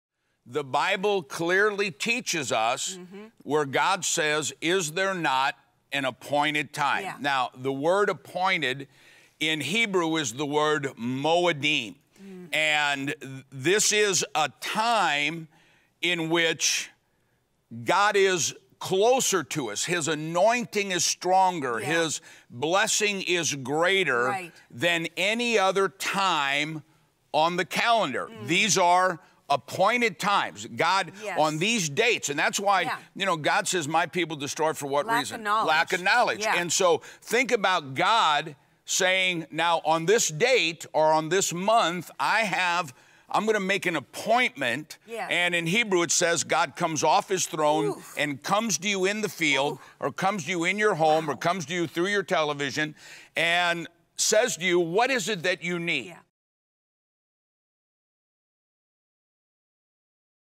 Is There Not An Appointed Time
The Word of God declares that there are appointed times, or moadims on God’s calendar.  They are times when God is closer than at any other time.  Three occur during the feasts of Passover, Pentecost and the Fall Feast of Sukkot.  During these times God opens a window of heaven and pours out a blessing that cannot be contained.